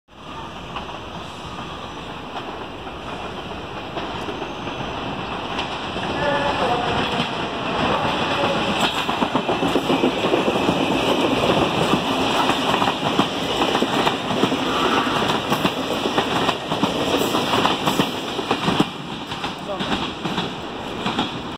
Toshiba Cabina 19 por estación Banfield
Formación eléctrica Toshiba Cabina 19 de Trenes Argentinos pasando por la estación Banfield con un servicio directo a estación Glew.